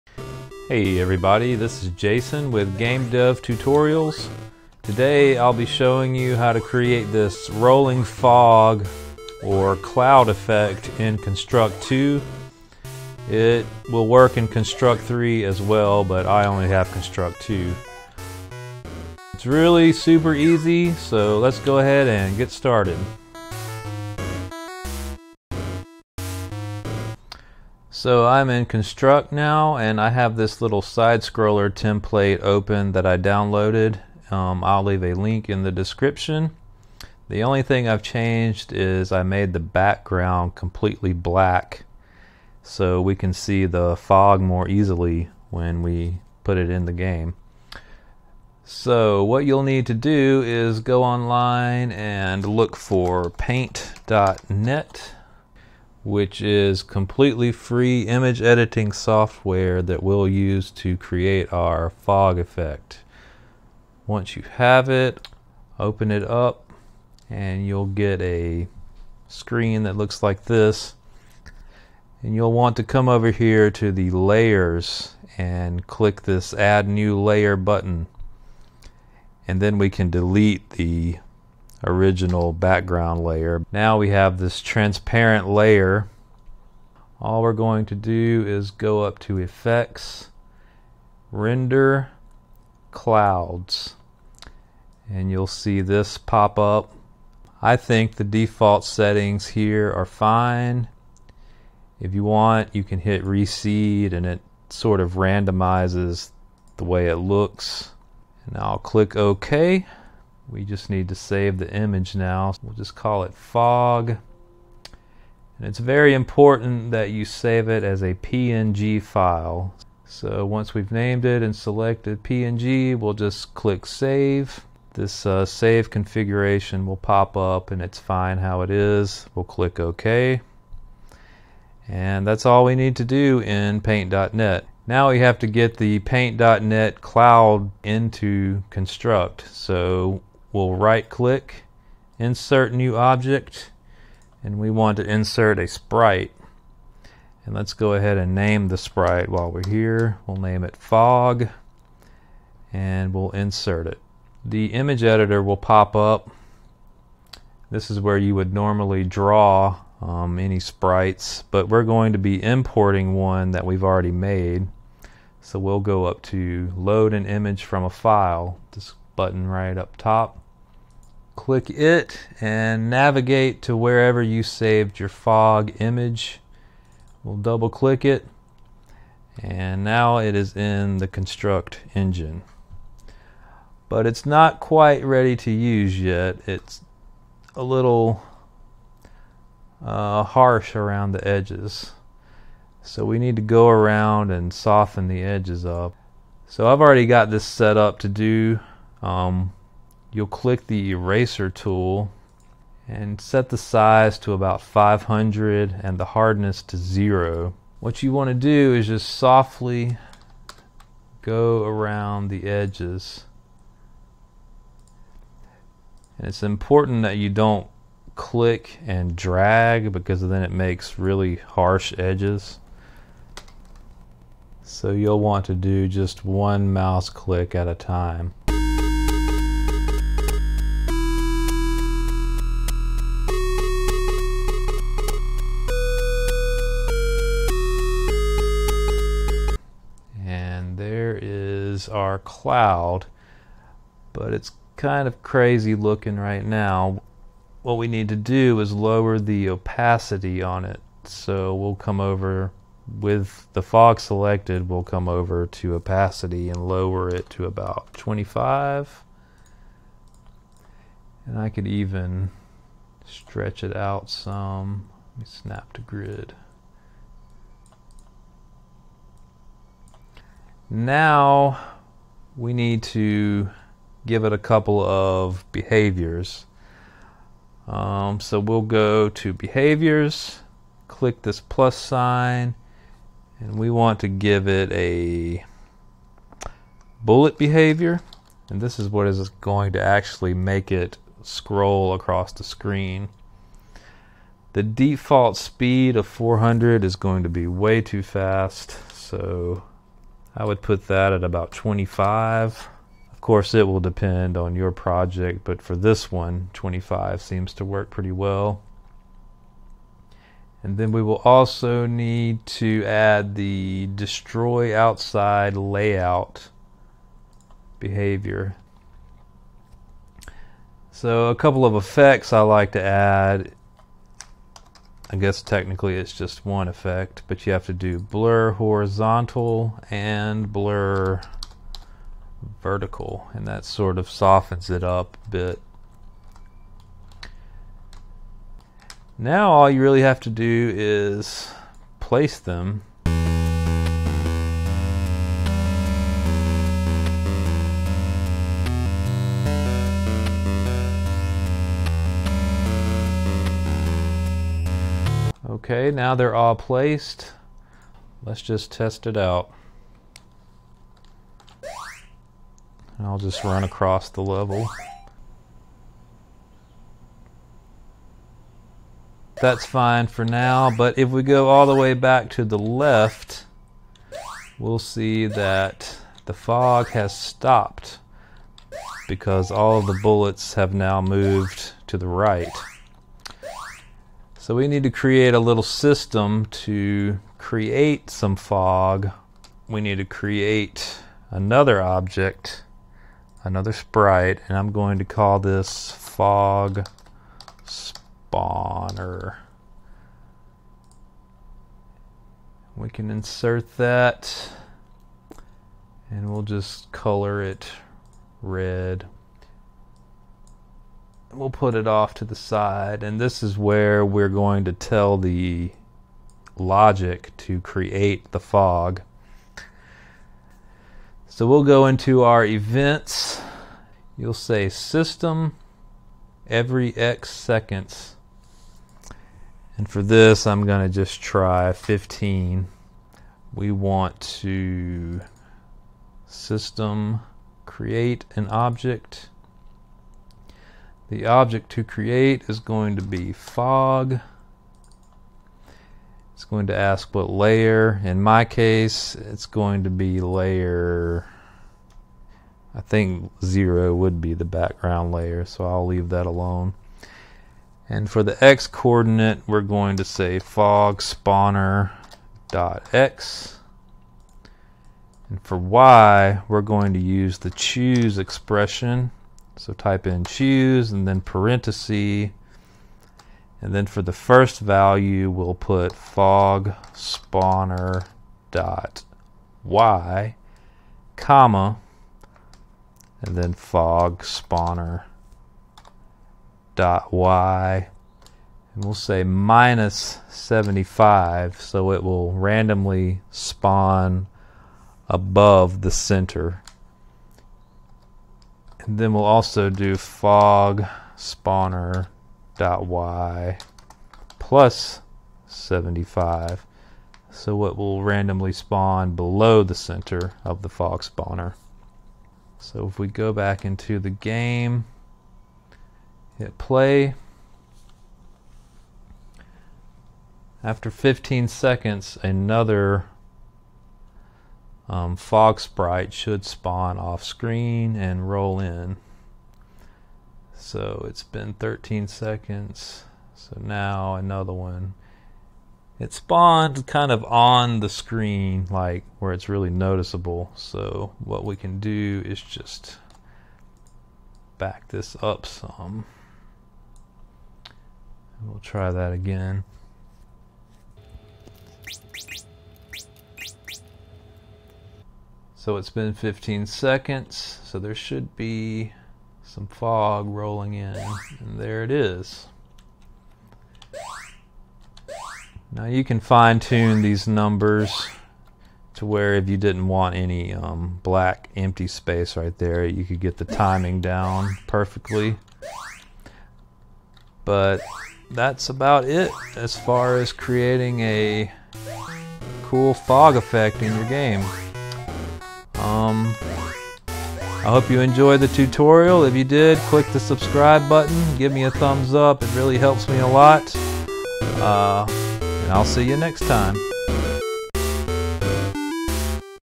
FOG or CLOUDS Effect in Construct 2 or Construct 3!!!
Learn how to create fog and/or clouds that move across the screen in Construct 2 or 3. All you need is Paint.net, which is free.